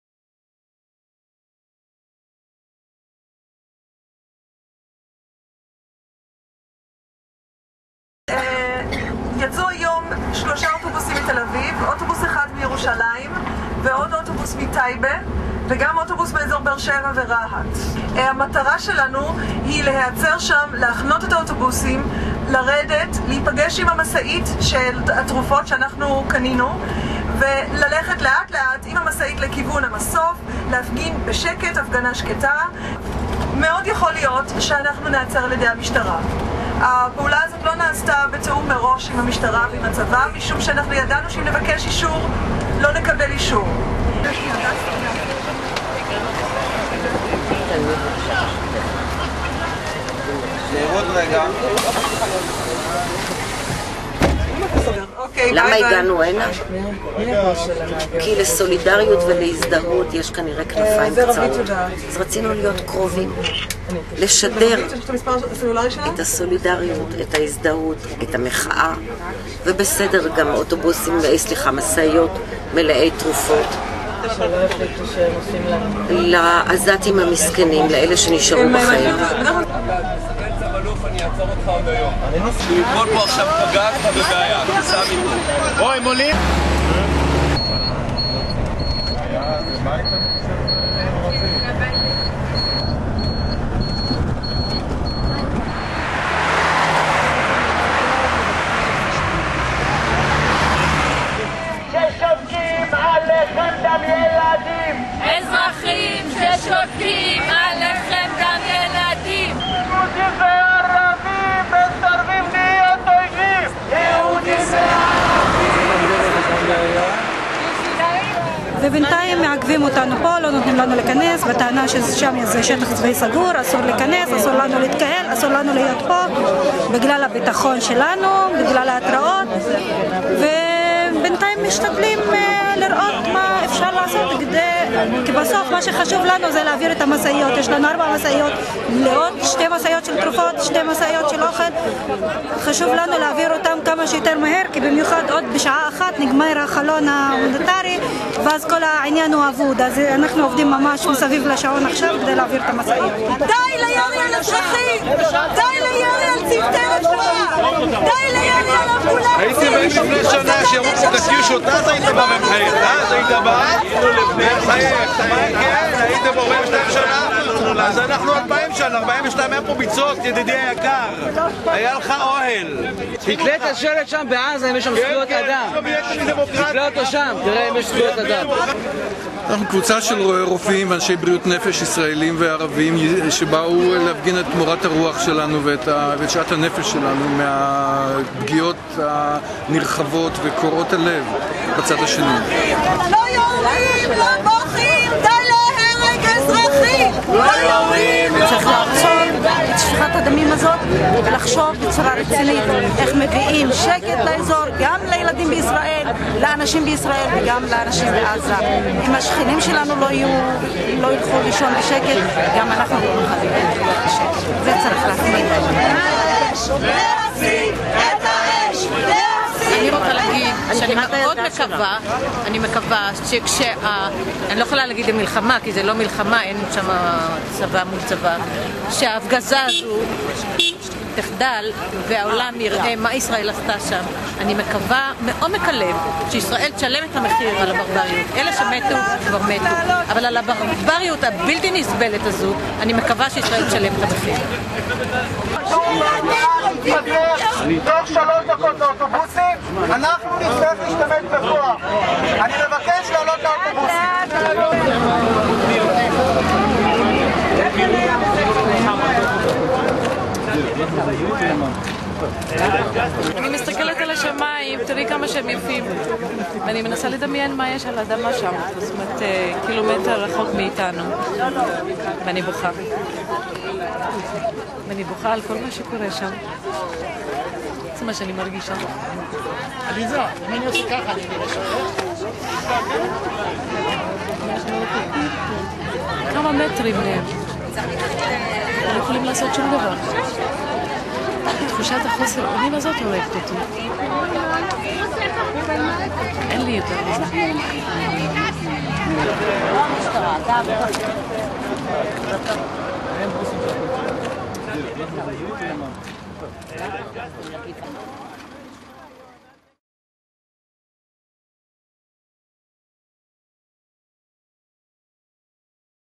Protest, Solidarity and Aid Convoy to Gaza
Israel Social TV presents:
Video, Edit & Translation: Dalit Baum.

Physicians for Human Rights - Israel,
January 16, 2009, during the Israeli attack

הטלוויזיה החברתית מציגה:
עזה - שיירת מחאה, סולידאריות וסיוע
צילום, עריכה ותרגום: דלית באום

רופאים לזכויות אדם - ישראל,
16 לינואר 2009, במהלך המתקפה הישראלית